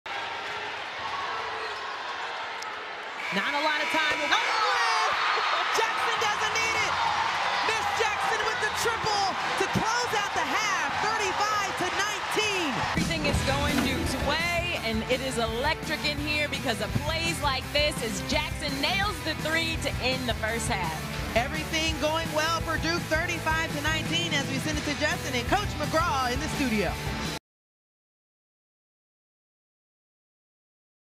Duke's Ashlon Jackson Closes Out The First Half With A 3
Everything seemed to go Duke's way in the first half against #6 NC State, and that included the way they closed out the half. Ashlon Jackson caught a pass on the wing with a second to go, and she drained a three-pointer to put the Blue Devils up by 16 at halftime.